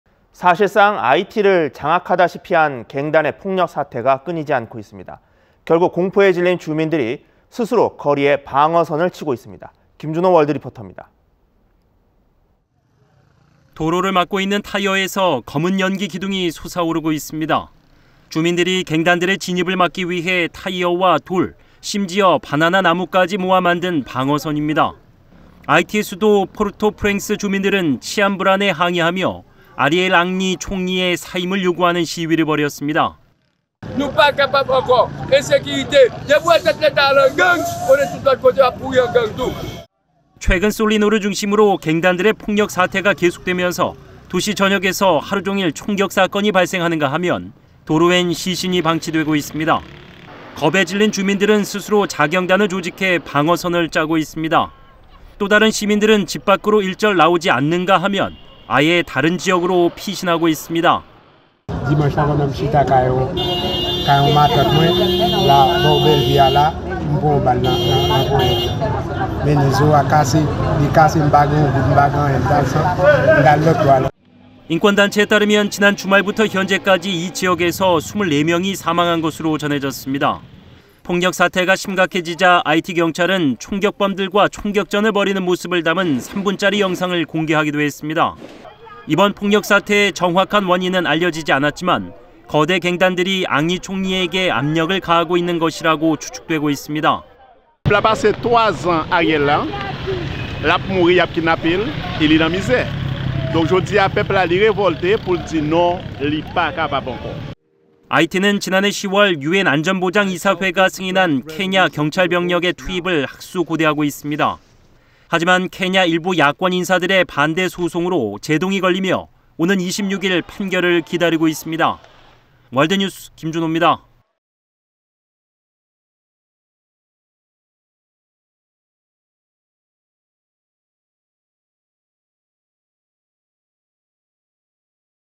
아이티, 주민이 갱단막으려 방어선구축
【앵커】
사실상 아이티를
장악하다시피한
갱단의 폭력 사태가
끊이지 않고 있습니다.
결국 공포에 질린
주민들이 스스로 거리에
방어선을 치고
있습니다.
 김준호 월드리포터입니다.

【기자】

도로를 막고 있는 타이어에서 검은 연기 기둥이 솟아오르고 있습니다.

주민들이 갱단들의 진입을 막기 위해 타이어와 돌, 심지어 바나나 나무까지 모아 만든 방어선입니다.

아이티 수도 포르토프랭스 주민들은 치안 불안에 항의하며 아리엘 앙리 총리의 사임을 요구하는 시위를 벌였습니다.

[르네 아우구스트 / 시위 참여자 : 우리는 더 이상 갱단 폭력에 따른 치안 불안을 견딜 수 없습니다. 국가원수는 이제 갱단에까지 연루되어 있고, 국가 전체가 갱단이 되어가고 있습니다. ]

최근 솔리노를 중심으로 갱단들의 폭력 사태가 계속되면서 도시 전역에서 하루 종일 총격 사건이 발생하는가 하면, 도로엔 시신이 방치되고 있습니다.

겁에 질린 주민들은 스스로 자경단을 조직해 방어선을 짜고 있습니다.

또 다른 시민들은 집 밖으로 일절 나오지 않는가 하면, 아예 다른 지역으로 피신하고 있습니다.

[네넬 돌메 / 갱단 폭력 피해자 : 일요일에 친구와 집 근처에 앉아 있었는데 총알 세 발이 날아왔어요. 한 발을 맞아 뼈가 으스러졌는데 병원에 갈 방법이 없어요.]

인권단체에 따르면 지난 주말부터 현재까지 이 지역에서 24명이 사망한 것으로 전해졌습니다.

폭력 사태가 심각해지자 아이티 경찰은 총격범들과 총격전을 벌이는 모습을 담은 3분짜리 영상을 공개하기도 했습니다.

이번 폭력 사태의 정확한 원인은 알려지지 않았지만, 거대 갱단들이 앙리 총리에게 압력을 가하고 있는 것이라고 추측되고 있습니다.

[레너드 진 레이날도 / 갱단 폭력 피해자 : 현 총리 집권 3년 동안 국민들은 납치, 살해, 빈곤으로 고통받았습니다. 이제 더 이상 참을 수 없다고 반발하고 있어요. ]

아이티는 지난해 10월 유엔 안전보장이사회가 승인한 케냐 경찰병력의 투입을 학수고대하고 있습니다.

하지만 케냐 일부 야권 인사들의 반대 소송으로 제동이 걸리며, 오는 26일 판결을 기다리고 있습니다.

월드뉴스 김준호입니다.